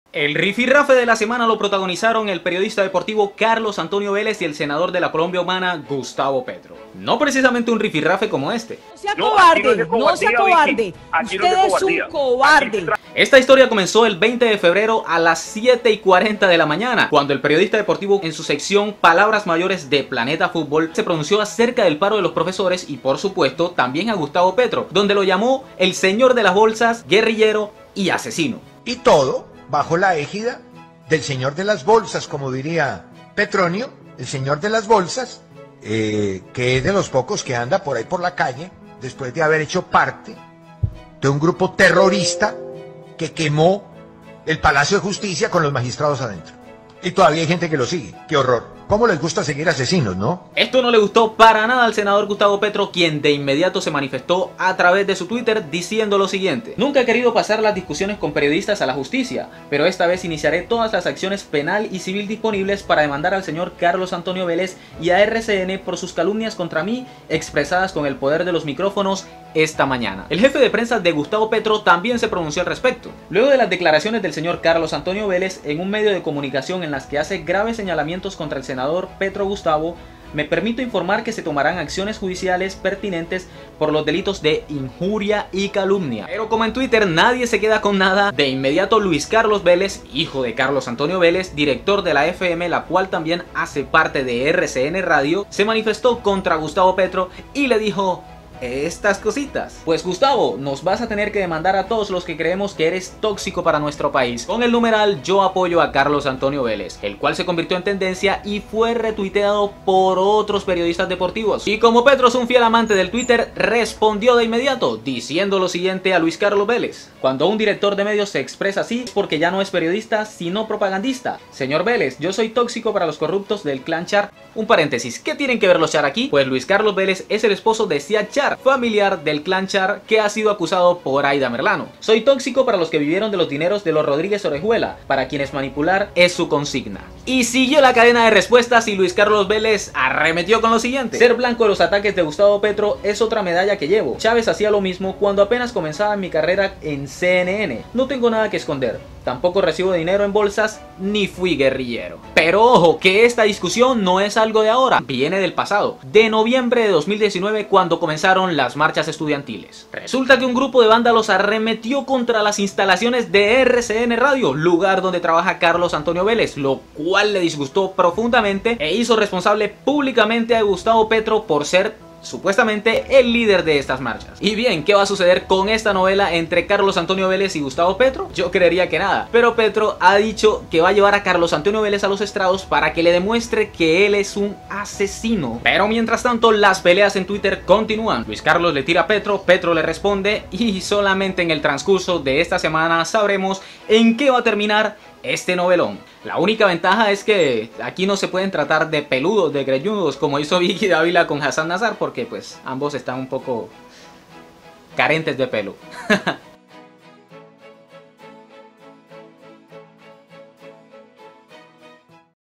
Carlos Antonio Vélez vs Gustavo Petro I EL RIFIRAFE DE LA SEMANA
El rifirafe de la semana lo protagonizaron el periodista deportivo Carlos Antonio Vélez y el Senador Gustavo Petro. ¿Cómo inició esta novela? En La Fecha te lo contamos.